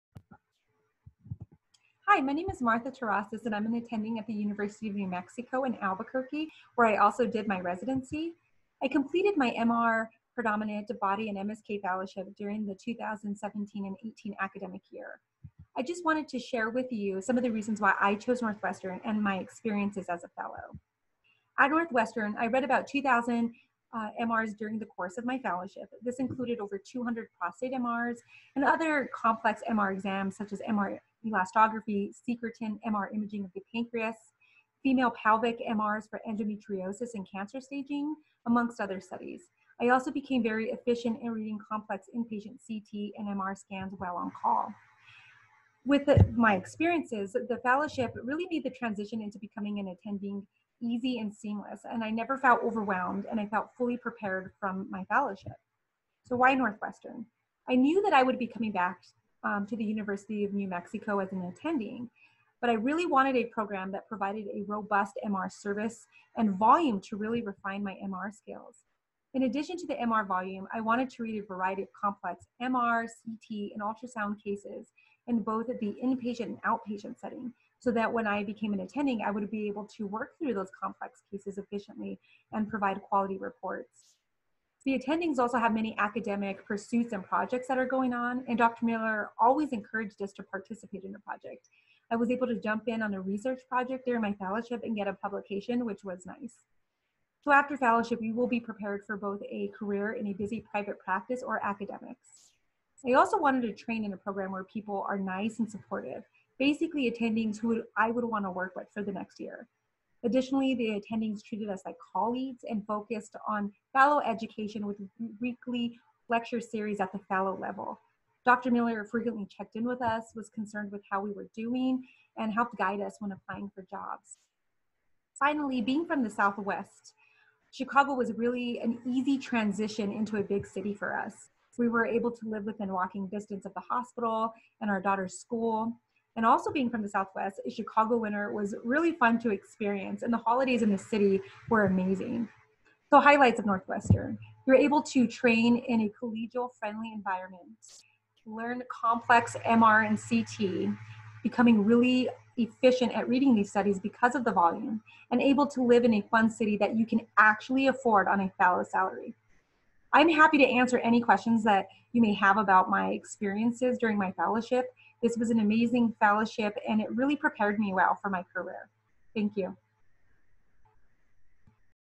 Martha Terrazas, MD
"Really made the transition to becoming an attending easy and seamless." Dr Martha Terrazas describes her Northwestern MR predominant body imaging fellowship experience (class of 2018, now University of New Mexico faculty, where she also did radiology residency).